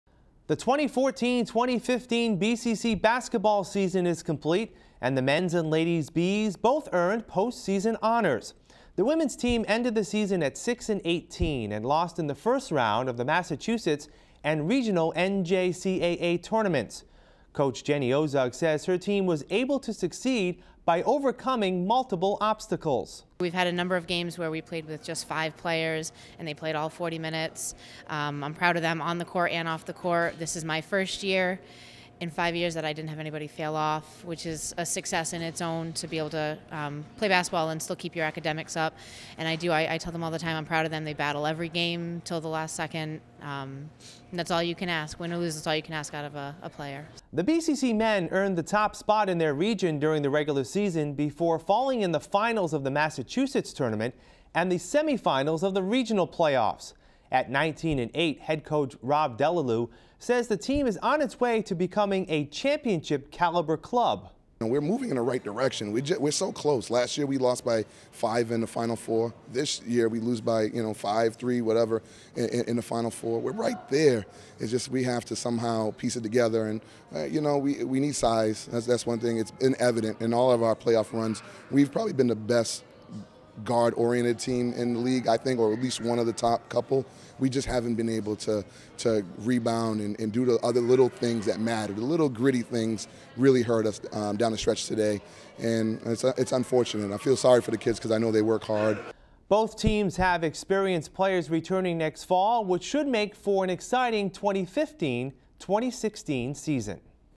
2015 BCC Basketball Review
2015 BCC Basketball Review
The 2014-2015 BCC Basketball season is complete and the men’s and ladies Bee’s both ear post season honors. The women’s team ended the season at 6 and 18 and lost in the first round of the MA and regional NJCAA tournaments. Coach Jenny Ozug says her team was able to succeed by overcoming multiple obstacles.
We have had a number of games where we played with just 5 players and they played all 40 minutes. I am proud of them on the court and off the court.  This is my first year in 5 years that I didn’t have anyone fail off, which is a success in its own to be able to play basketball and keep your academics up.  And I do, I tell them all the time I am proud of them. They battle every game till the last second. And that is all you can ask.  Win or lose that is all you can ask out of a player.
The BCC men earned the top spot in their region during the regular season before falling in the finals of the MA tournament.  And the semi-finals of the regional playoffs. At 19 and 8, the head coach, Rob Delalu says the team is on its way to becoming a championship caliber club.
We are moving in the right direction. We are so close. Last year we lost by 5 in the final 4.  This year we lose by 5, 3 whatever in the final 4.  We are right there.  It’s just we have to somehow piece it together. We need size.  That is one thing. It’s evident in all of our playoff runs. We probably have been the best guard oriented team in the league I think or at least one of the top couple. We just haven’t been able to rebound and due to other things that matter, the little gritty things really hurt us down the stretch today. And it’s unfortunate. And I feel sorry for the kids because I know they work hard.
Both teams have experienced players returning next fall. Which should make for an exciting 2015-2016 season.